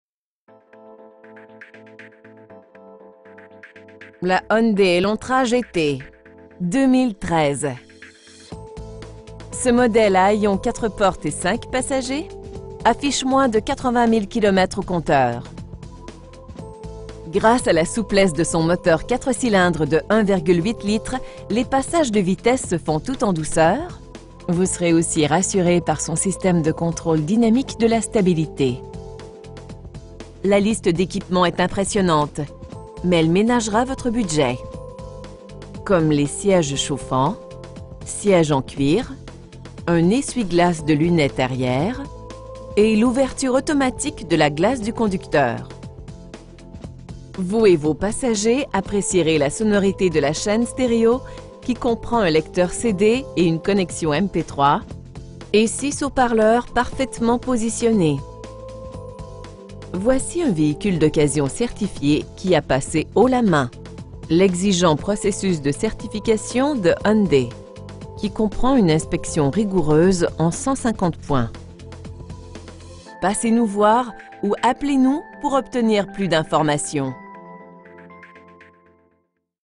2013 Hyundai Elantra GT SE AUTO A/C TOIT PANORAMIQUE CUIR
Brossard Hyundai
8750, Boul Taschereau

Sans complexe et pratique, voici la Hyundai Elantra GT 2013. Ce mod��le �� hayon 4 portes et 5 passagers affiche moins de 80 000 km au compteur. Gr��ce �� la souplesse de son moteur 4 cylindres de 1,8 litres, les passages de vitesses se font tout en douceur, vous serez aussi rassur�� par son syst��me du contr��le dynamique de la stabilit��. La liste d'��quipement est impressionnante, mais elle m��nagera votre budget. Comme les si��ges chauffants, si��ges en cuir, un essuie-glace de lunette arri��re, une t��l��commande int��gr��e d'ouvre-porte de garage, le r��troviseur central avec fonction anti��blouissement automatique, un afficheur de la temp��rature ext��rieure, phares �� contr��le automatique, et l'ouverture automatique de la glace du conducteur. L'habitacle propose de nombreuses solutions de rangement qui sont le reflet d'un souci ��lev�� du d��tail. Le toit ouvrant �� commande ��lectrique permet de se rapprocher de la nature et d'humer l'air frais en un tour de main. Vous et vos passagers appr��cierez la sonorit�� de la cha��ne st��r��o, qui comprend une lecteur CD et une connexion MP3, commandes audio sur le volant, et 6 haut-parleurs parfaitement positionn��s. Hyundai accorde la priorit�� �� la s��curit�� et la protection en incluant: coussins gonflables �� l'avant, rideaux gonflables pour prot��ger la t��te, syst��me antipatinage, syst��me d'assistance au freinage, un syst��me antivol, et freins �� disque aux 4 roues avec antiblocage. Voici un v��hicule d'occasion certifi�� qui a pass�� haut la main l'exigent processus de certification de Hyundai, qui comprend une inspection rigoureuse en 150 points! Venez profiter d'une agr��able exp��rience d'achat: nous avons de l'humour et nous offrons toute l'information qui vous permettra de prendre une d��cision ��clair��e, sans pression. Passez nous voir ou appelez-nous pour obtenir plus d'information.